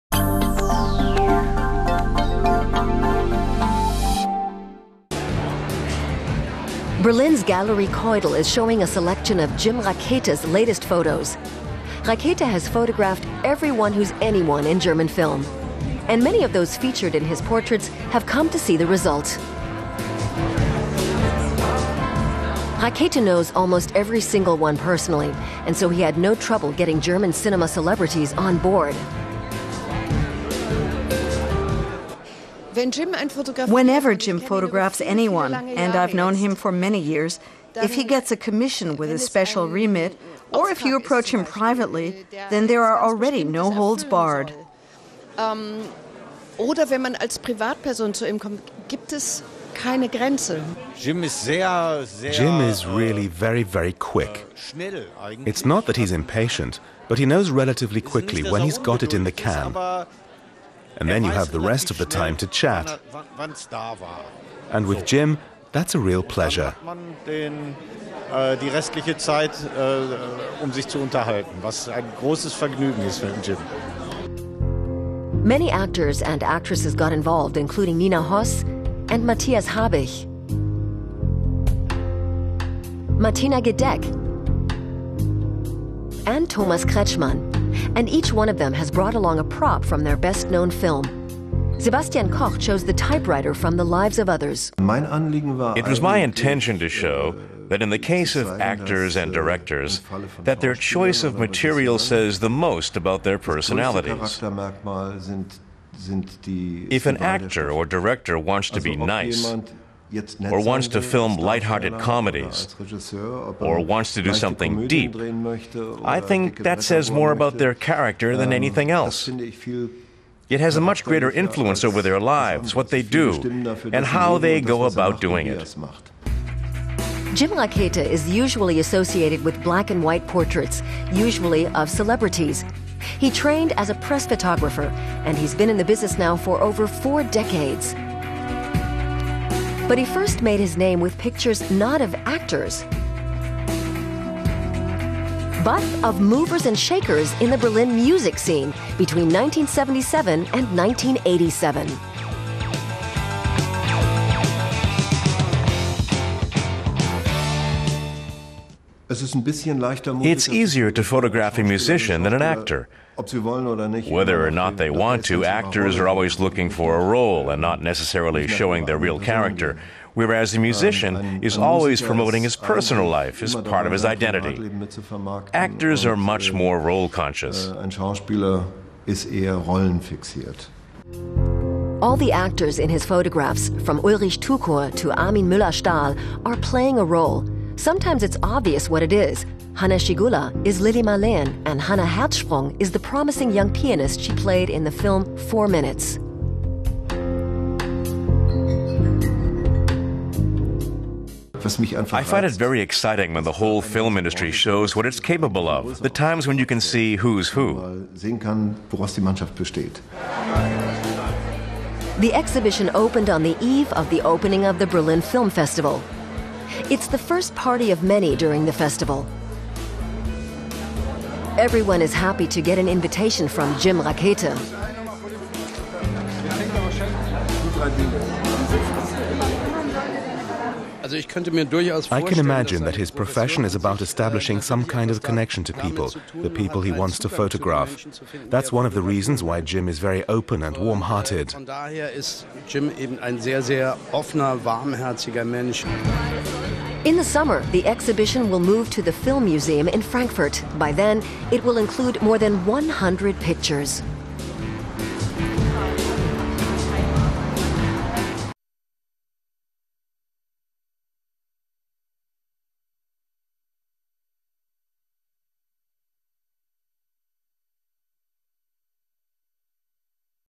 Celluloid close-ups | Video of the day
The latest project of Berlin-based photographer Jim Rakete involves 100 portraits of German film celebrities. The photos will go on show when the Deutsches Filmmuseum reopens in the summer.